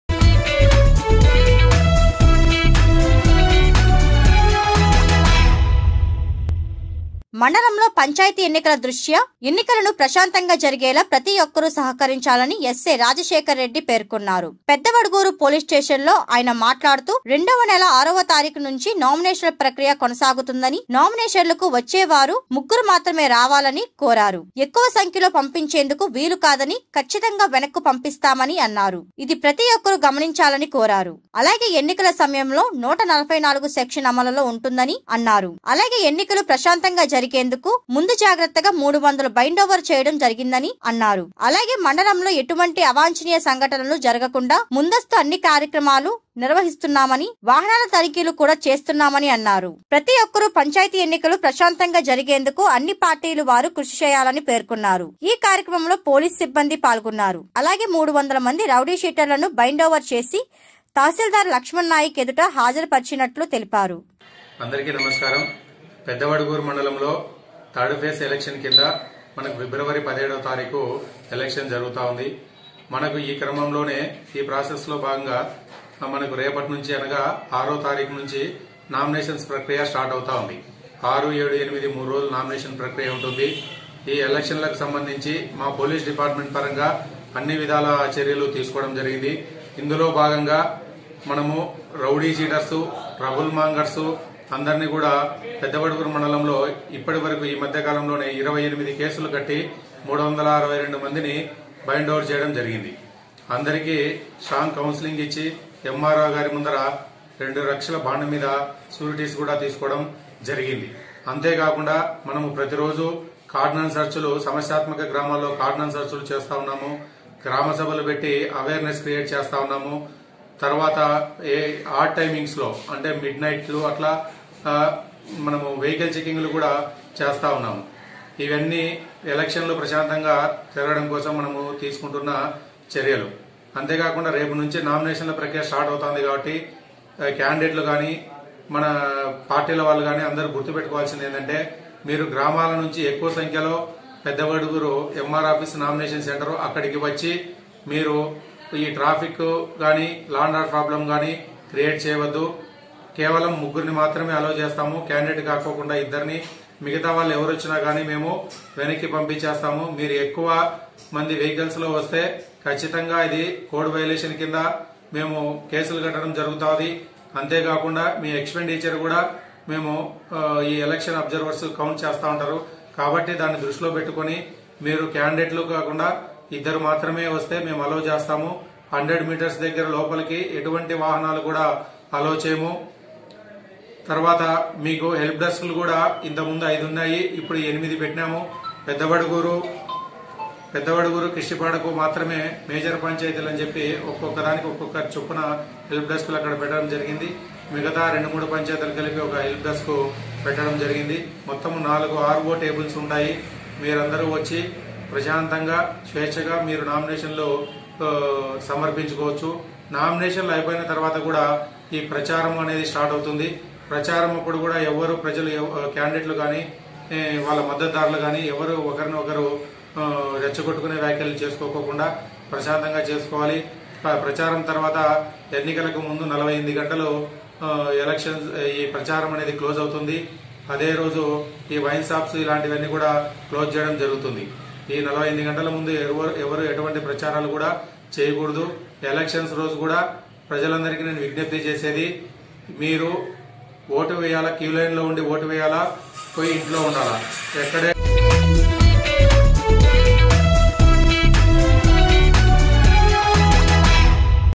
పెద్దవడుగూరులో ఎన్నికలు ప్రశాంతంగా జరిగేలా ప్రతి ఒక్కరు సహకరించాలి...SI రాజశేఖర్ రెడ్డి...
@truthnewstelugu Everyone should cooperate for the peaceful conduct of elections in Paddavaduguru ... SI Rajasekhar Reddy.